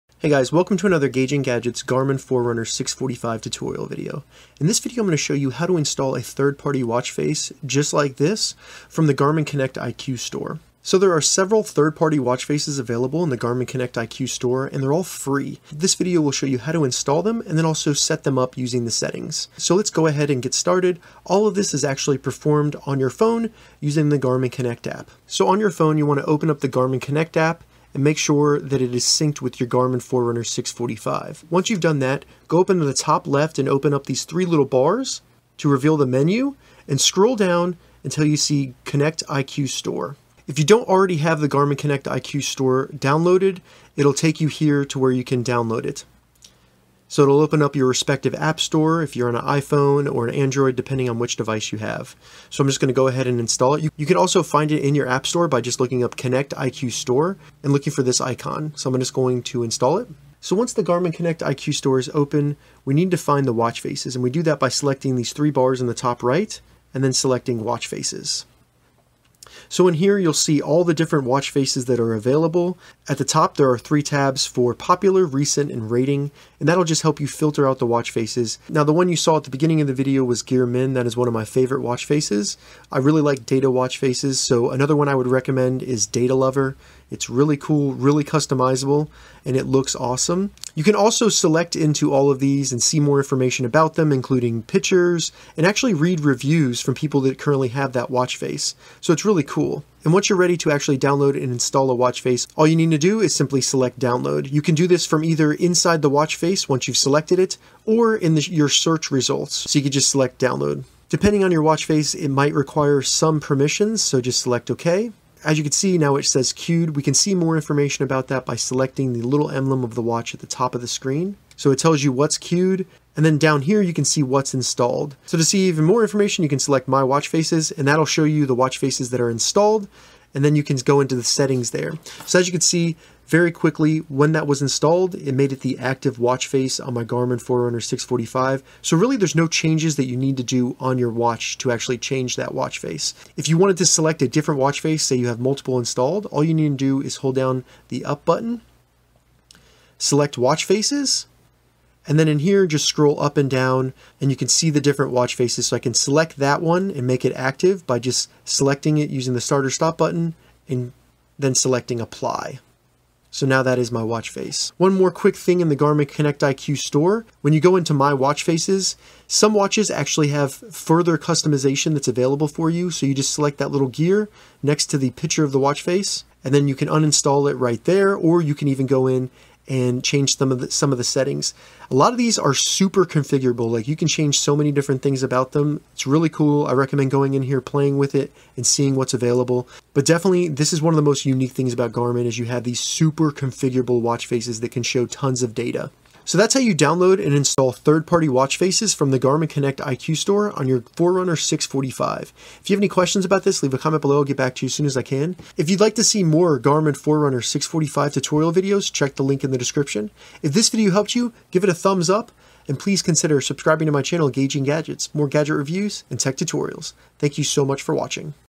How to Install Watch Faces - Garmin Forerunner 645 Tutorial
How to install watch faces on your Garmin Forerunner 645 or Garmin Forerunner 645 Music. In this tutorial, I show you how download watch faces from the Connect IQ store and install them on your Garmin Forerunner 645. The Garmin Forerunner 645 comes with several default watch faces, but the Garmin Connect IQ store offers a wide variety of awesome watch faces.
Garmin Forerunner 645 Music on Amazon
Garmin Forerunner 645 on Amazon

Useful Garmin Forerunner 645 Accessories 👇
Extra Charging Cables
Screen Protectors
Replacement Watch Bands

Many of the watch faces in the Connect IQ store offer the following data sets: Battery, Calendar, Calories, Date, Floors, Garmin, Intensity Minutes, Move Bar, Seconds, Status, Steps, Sunrise/Sunset, and Weather. You can also get novelty watch faces that look similar to  military designs, popular movies, sports teams, or characters.

Some of the most popular watch faces are Data Lover, Actiface, Gearmin, NoFrills, Steam Gauge, Simple Info Analog, Digic Watch, Sport Master, and Titanium.

How to Install Watch Faces on Garmin Forerunner 645/Music?

Install Garmin Forerunner 645 Watch Faces:
1. Open the Gamin Connect app and select the Menu button located in the top-left of the app.
2. Once the menu opens, select Connect IQ Store.
3. Login to the Garmin Connect IQ Store.
4. Once the store loads, select the blue drop-down menu labeled App Type and select Watch Faces.
5. Search through the available watch faces. Once you find a watch face for your Garmin Forerunner 645, select the watch face.
6. Once the watch information screen loads, select download, accept the terms, and select Allow.
7. The next time your watch syncs, your new watch face will be installed.
8. You have now installed a third party watch face on your Garmin Forerunner 645 or Garmin Forerunner 645 Music.